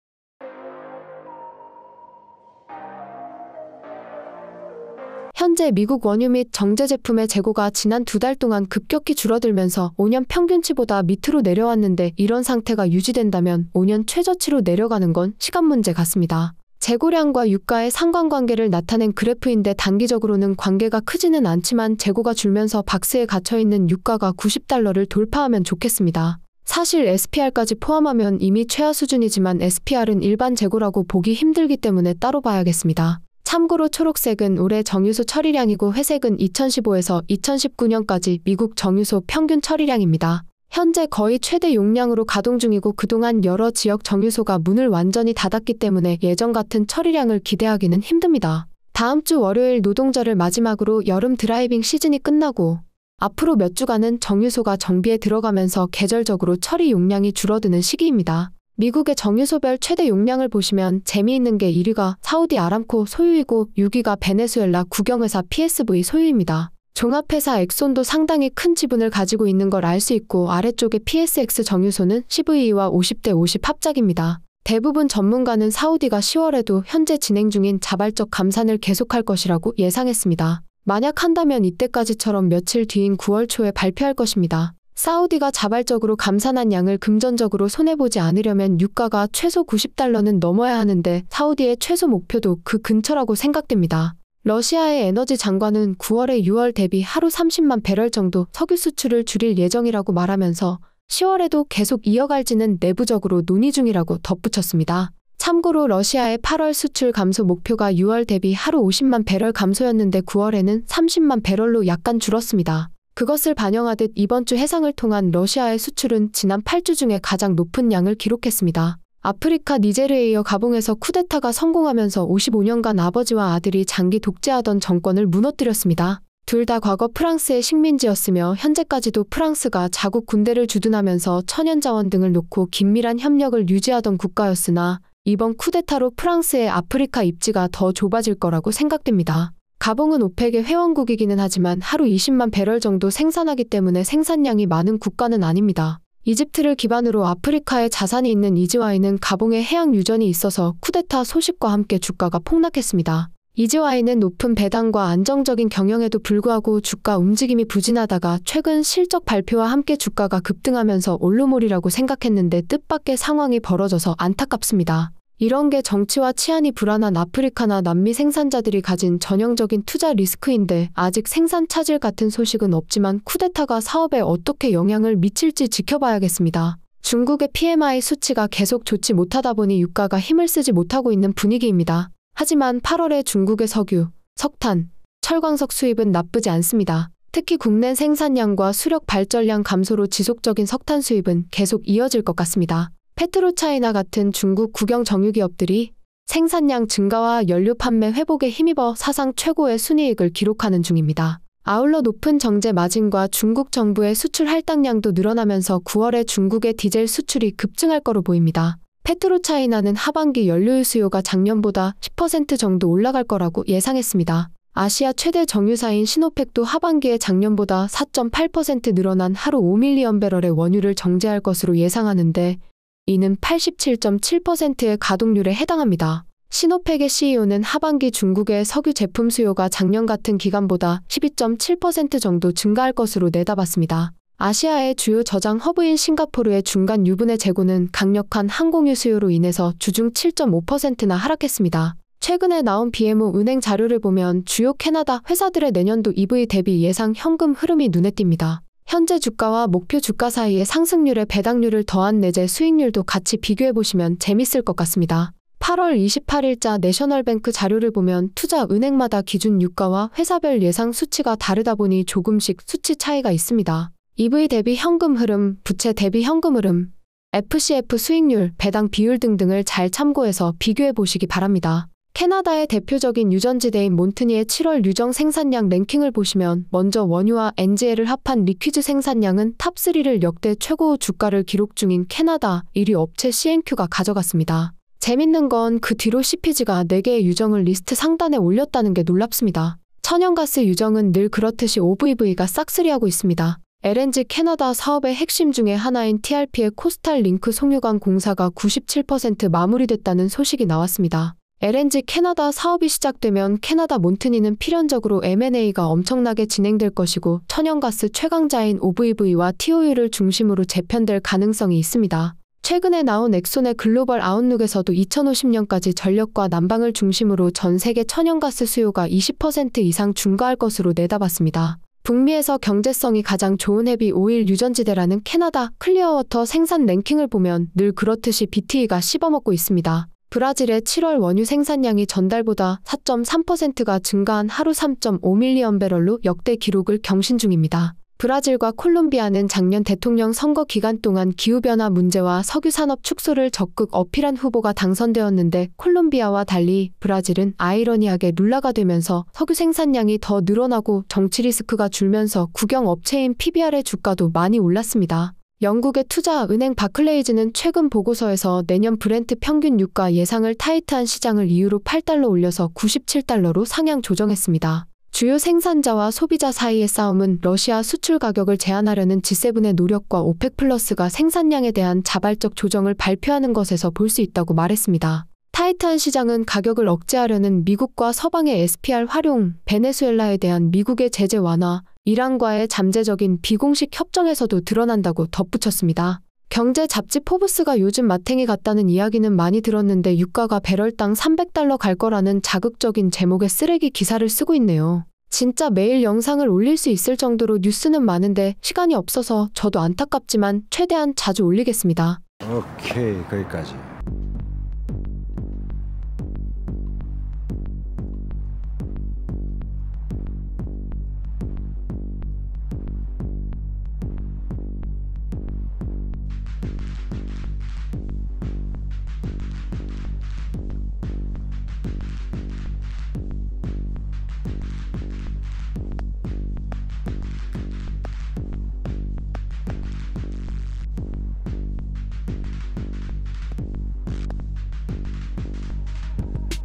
9월 1일 주식 뉴스 – 유가 배럴당 $300 갈 거라는..
안녕하세요. 미국 주식들을 소개하고 분석할 천수르입니다. 방문해주셔서 감사합니다! 영상 내용은 제 개인적인 의견이니 참고만 하시고 각자 공부하셔서 현명한 투자하시기 바랍니다. 투네이션 후원 링크 감사드립니다!

00:00 미국 상업 재고량
01:14 사우디와 러시아 10월에도 자발적 감산?
01:59 OPEC 회원국 아프리카 가봉 쿠대타 성공, EGY 위기
02:56 중국 경제와 석유/석탄 수요 그리고 정유 산업
04:02 BMO와 내셔널뱅크의 캐나다 석유 주식들 수치
04:32 캐나다 몬트니와 클리어워터 생산자 랭킹, CNQ, CPG, OVV, TOU, BTE, TRP
05:30 브라질 생산량 증가와 정치 리스크 감소, PBR
05:55 바클레이즈와 포브스의 유가 예측

* source : a company's website